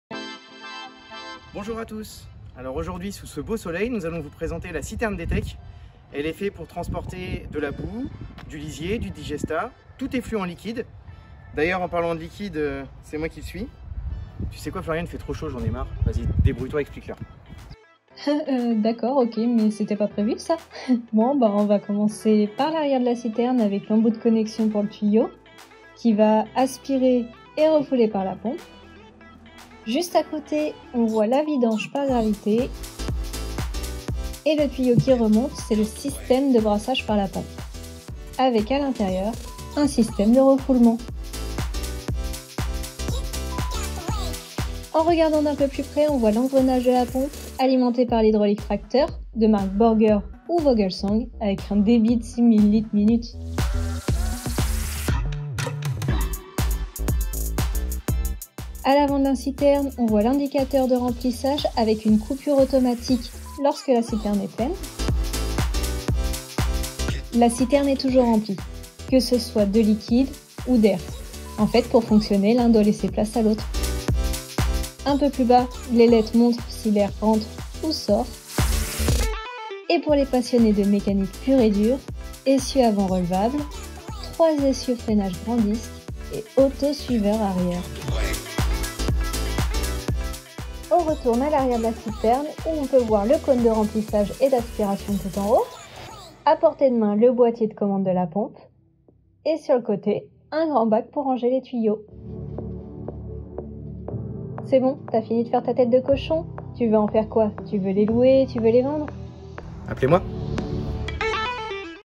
Présentation de notre Citerne DELTA🔺TRAILERS avec Vincent et Floriane (D-TEC)
Démonstration de notre citerne avec Vincent 💁🏻‍♂️et Floriane 👱🏻‍♀️
🔺 Disponible dés aujourd'hui

 Destinée au transport de :
🐮 Lisiers et autres digestats
🟤 Graisse, glycérine, boues de station
💧 Et bien d'autres effluents liquides !!

↗️ Légère
↗️ Efficace

➡️ Marque D-TEC
Des questions ❓
Posez-les dans les commentaires
OU
Contactez Vincent, notre commercial DELTA🔺TRAILERS :
📞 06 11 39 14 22